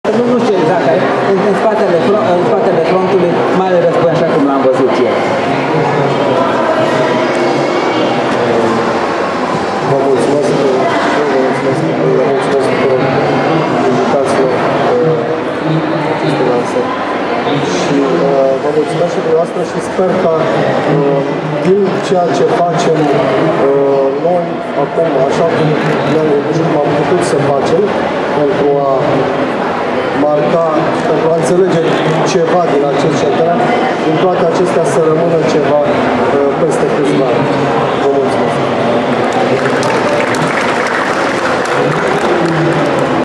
(6) Radu Gârmacea - Lansare două cărți pe tema Marelui Război (Bookfest, Humanitas, 2018.06.03)
În data de 3 iunie 2018 a avut loc la standul editurii Humanitas din cadrul Bookfest (Romexpo, București, 2018) lansarea cărților "Jurnalul unui medic militar, 1917–1918", de Raul Dona și "În spatele frontului. Marele Război, așa cum l-am văzut eu, decembrie 1916–iunie 1918. Însemnările unui soldat german în România ocupată", de Gerhard Velburg.

Vorbitori: Simona Tivadar, Ioana Pârvulescu, Viorica Milicescu, Ștefan Colceriu, Radu Gârmacea.

Mai jos, fotografii de la eveniment și înregistrarea video și audio a evenimentului.